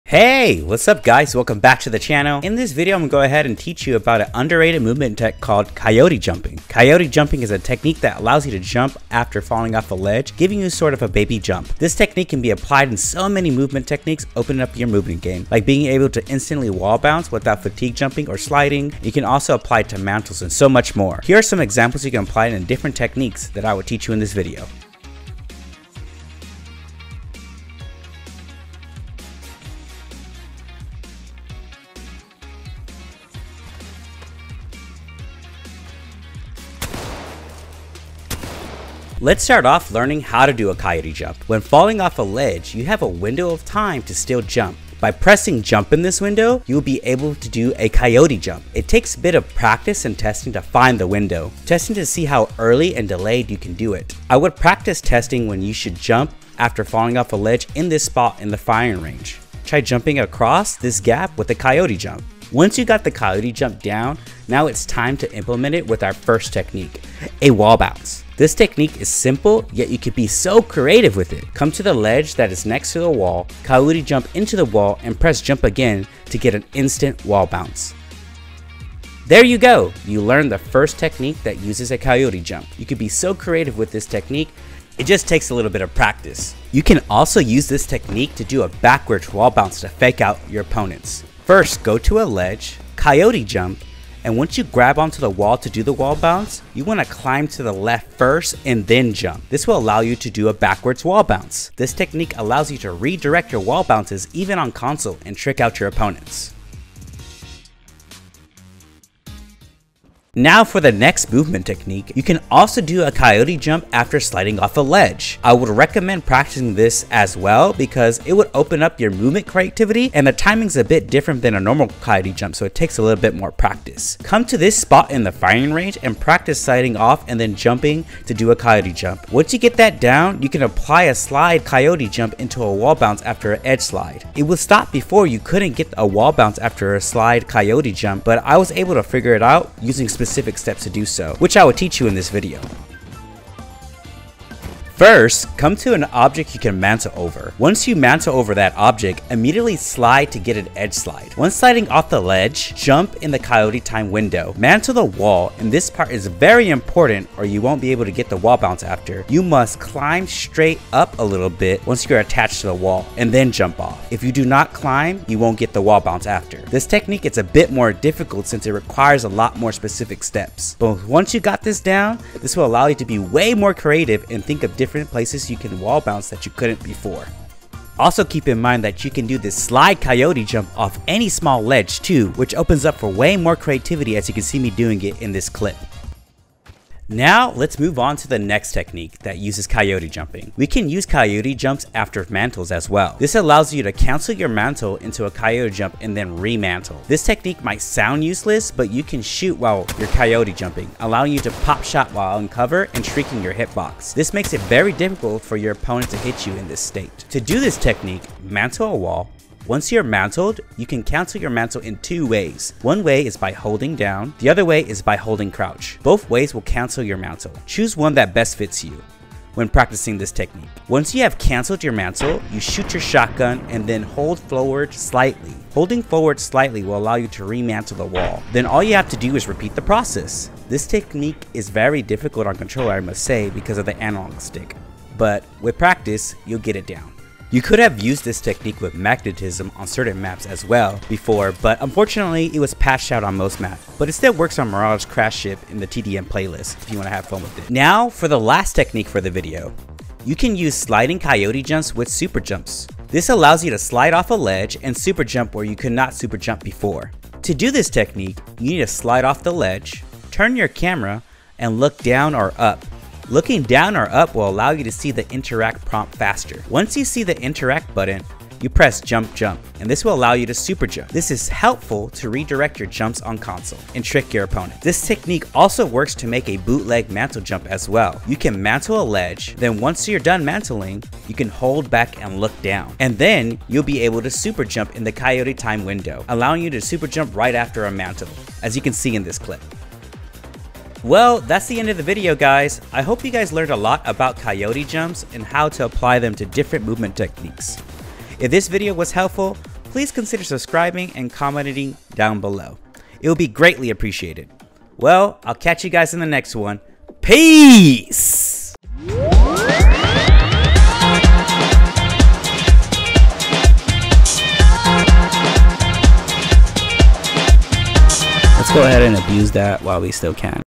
Movement Technique That Allows You To Jump Mid Air - How to Coyote Jump - (Underrated Tech)
Hey what's up dude and dudette. In this short tutorial, I teach you a technique that allows you  jump after you have fallen off a ledge, aka coyote jump. You can apply this coyote jump so many other techniques like  super jumps, wall bounces, edge slides, superglides and much more. Check out the video to up your movement game. Works both on PC/Console.